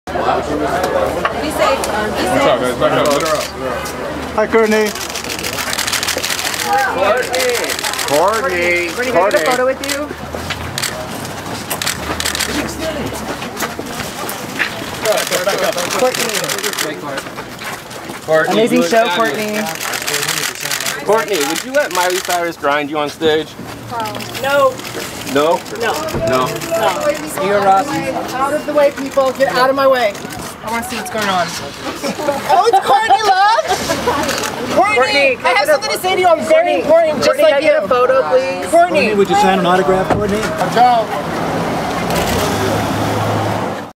Courtney Love no dejaría que Miley le bailara
Un paparazzi pregunta a Courtney Love si ella dejaría que Miley hiciera su famoso twerking con ella, y Love rápidamente responde que ¡NO!